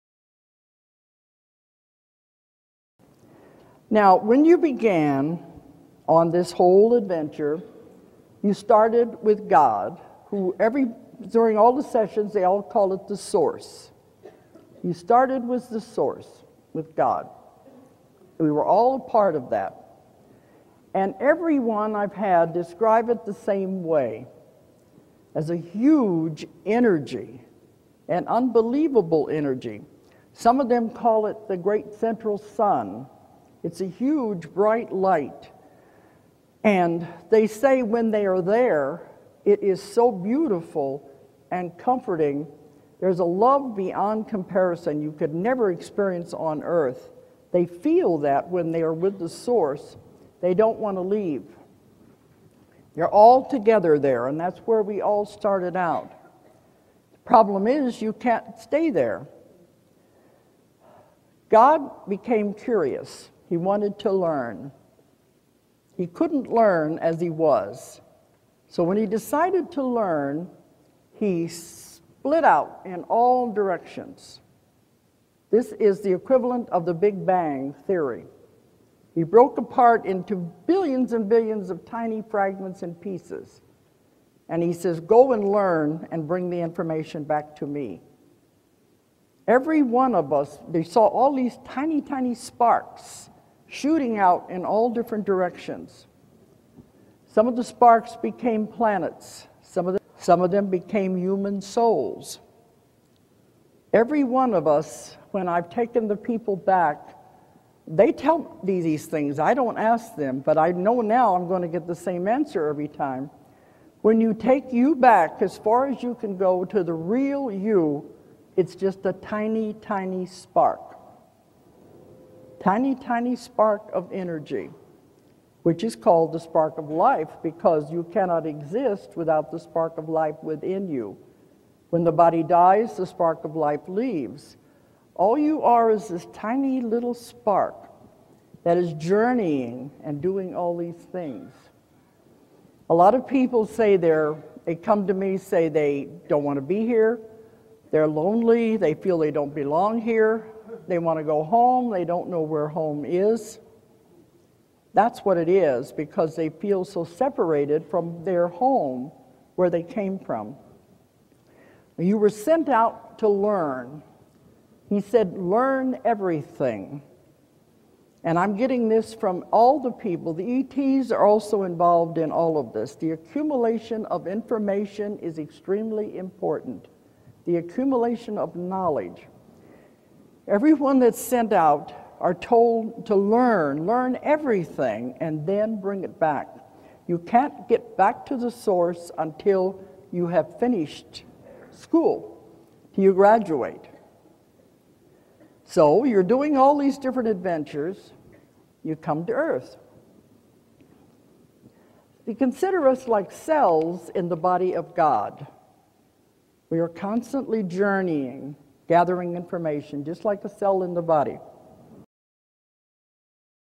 ~GOD BECAME CURIOUS~DOLORES CANNON
This is a short video where Dolores explains who we really are.